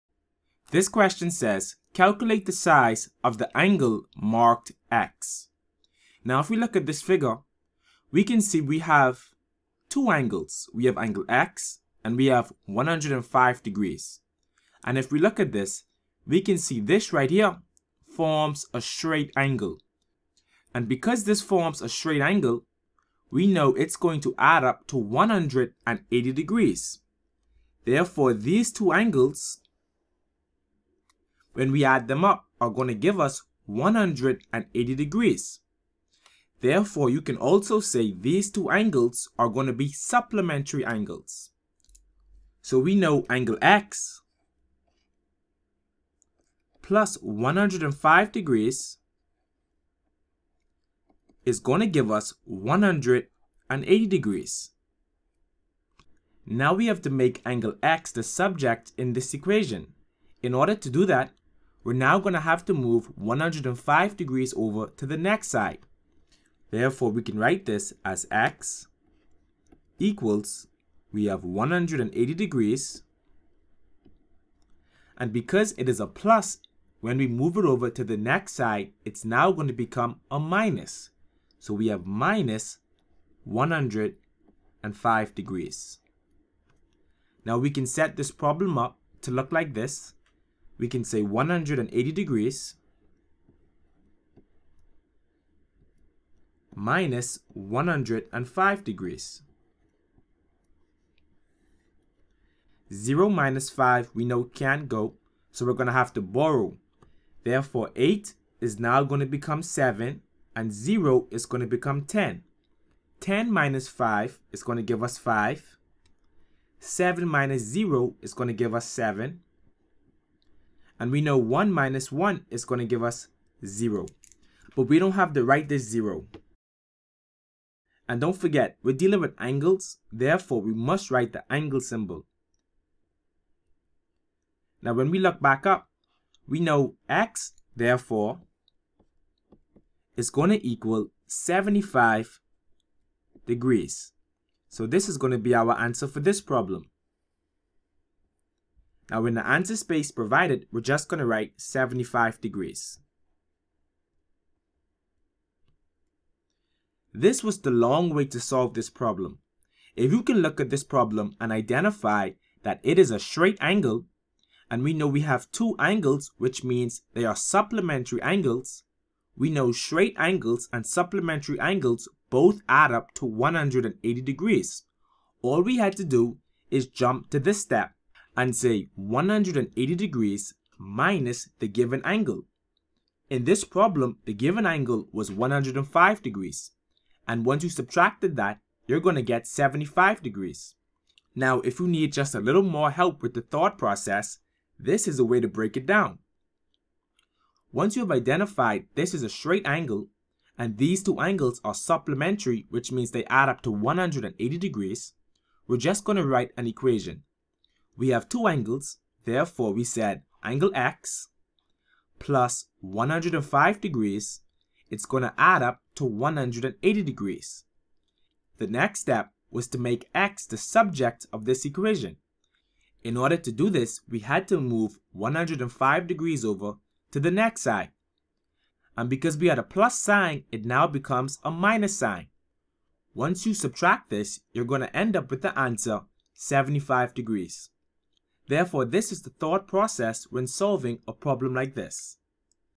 BJC Math Paper 2 - Solving a missing angle (Straight Angle / Supplementary Angle)
In this video, we learned how to calculate a missing angle. This is a straight angle and these angles are also supplementary, Therefore the total of these angles is going to add up to 180 degrees.